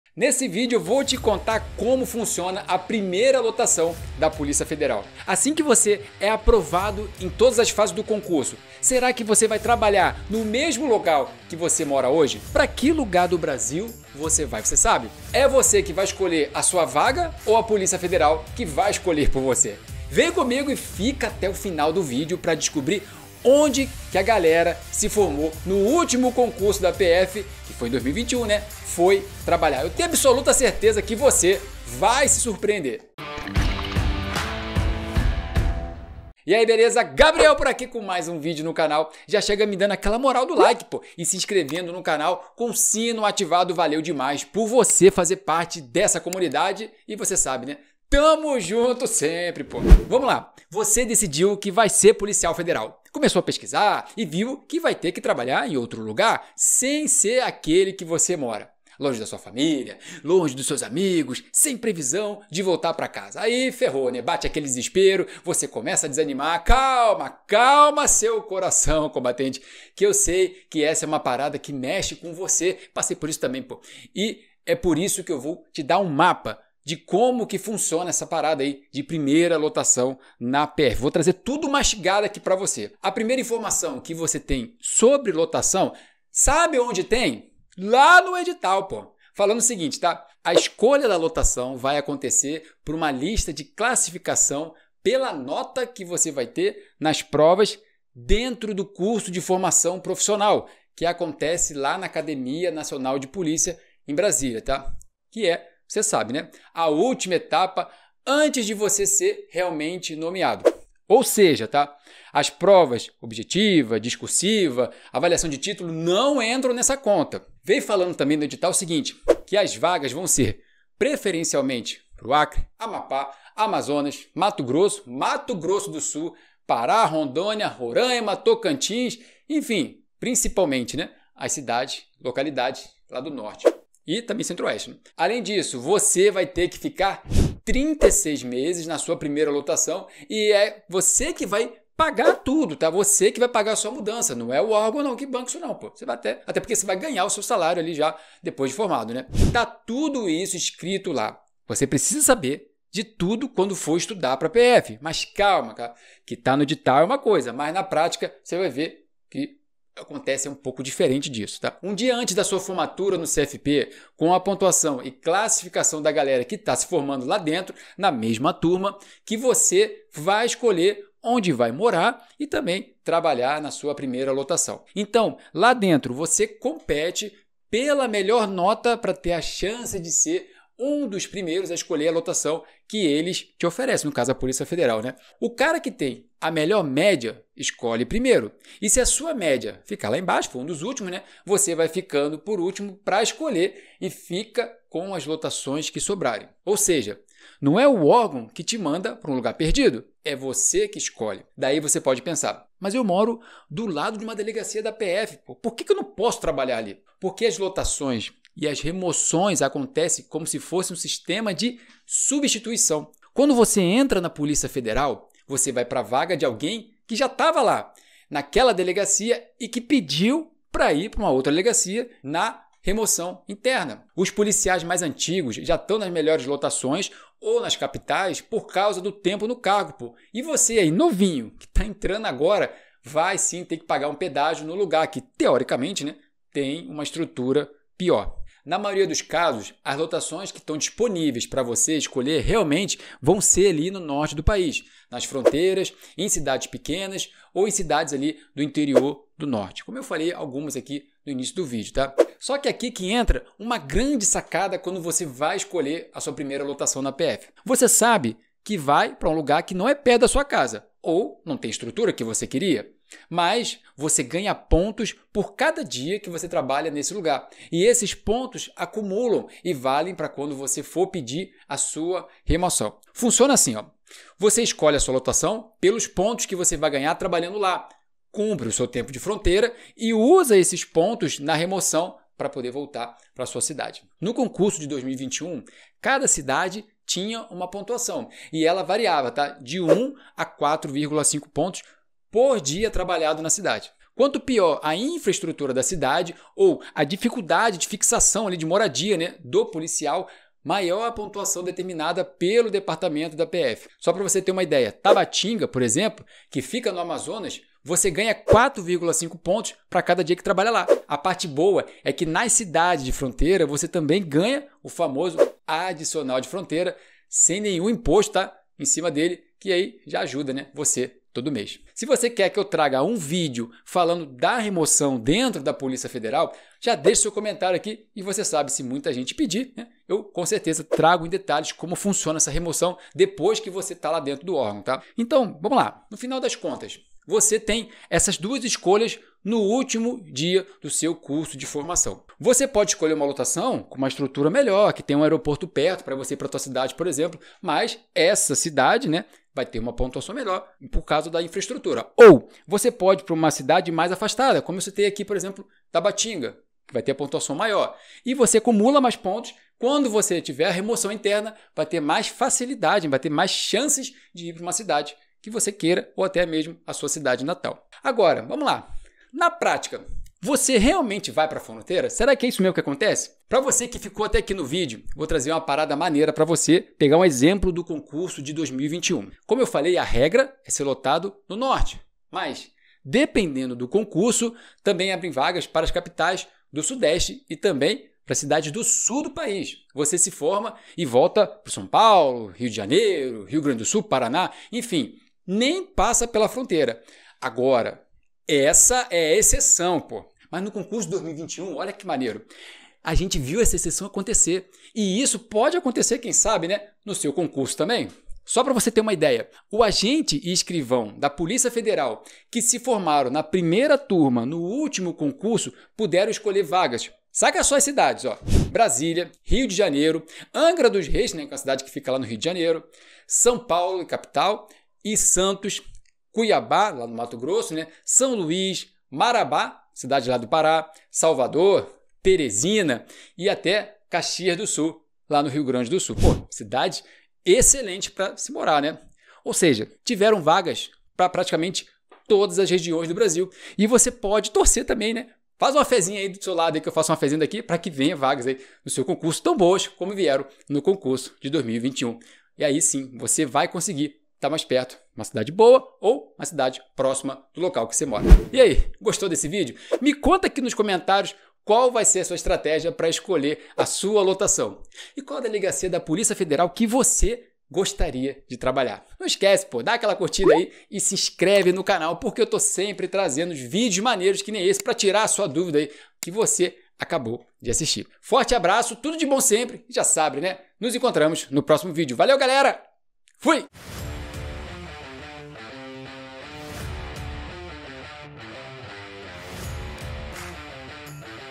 PRIMEIRA LOTAÇÃO PF: ONDE VOU TRABALHAR DEPOIS DA FORMATURA NA POLÍCIA FEDERAL?
Você sabe onde vai trabalhar depois da formatura da Polícia Federal? Vai para fronteira? Vai para os estados do Norte? Afinal, onde será a sua primeira lotação na PF?

Vem comigo que eu te explico em detalhes nesse vídeo.

Bora conferir?